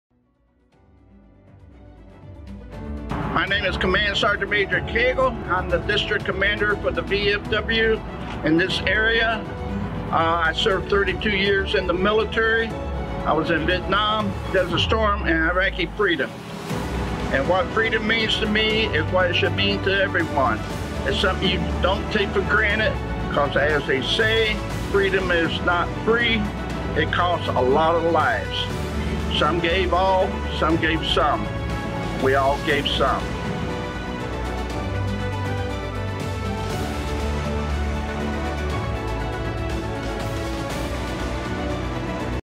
Thank You for Your Service - at Apple Chevrolet
At Apple Chevrolet we want to (R)emember (E)veryone (D)eployed. We would like to take a moment and say thank you to all who have served our great country.